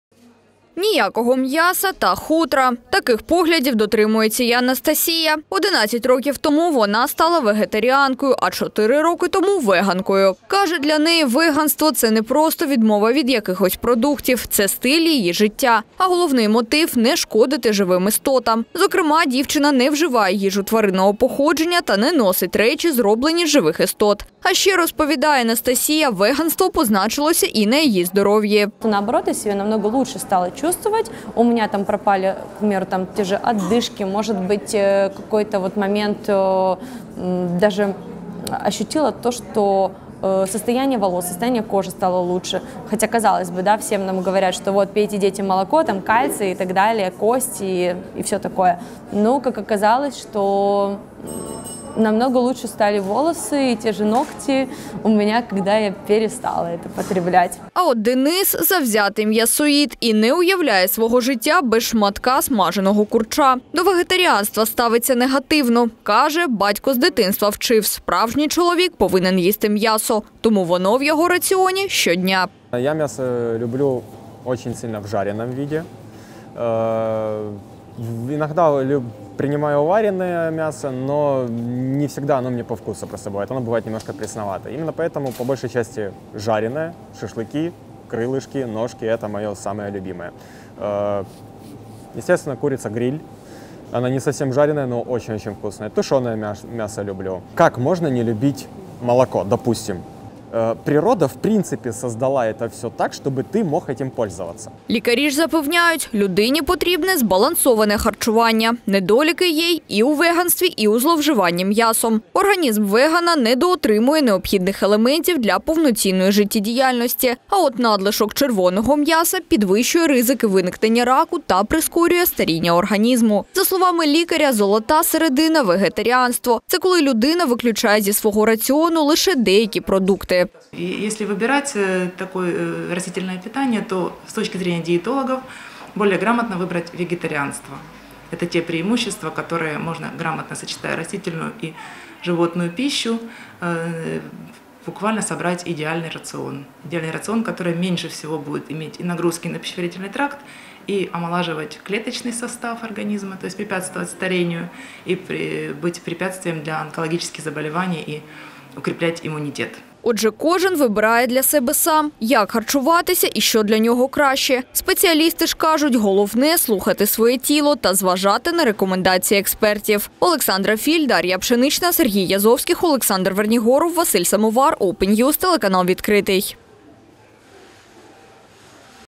Міжнародний день вегана: чи корисно бути веганом
Без смаженого курча та шкіряних чоботів. Так живуть вегани. Це люди взагалі не вживають їжу тваринного походження: м’ясо, молоко, яйця. А ще активно виступають проти вбивства тварин. Тому не носять хутряні та шкіряні вироби. Кожен з цих людей прийшов до такого життя через різні причини: від етичних, соціальних, моральних, фізичних до духовних. Яке воно життя без м’яса та чи корисно це для людини, до Міжнародного дня вегана дізнавалася Олександра Філь.